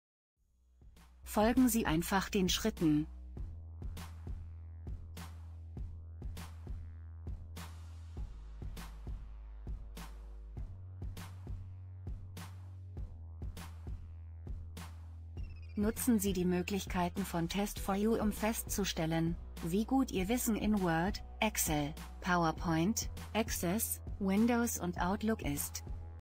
Stellen Sie sicher, dass nur der benannte Bereich Average_Questions angezeigt wird, wenn die...
Stellen Sie sicher, dass nur der benannte Bereich Average_Questions
angezeigt wird, wenn die Arbeitsmappe im Browser angezeigt wird.Möchten Sie mehr. Laden Sie das TEST4U demo herunter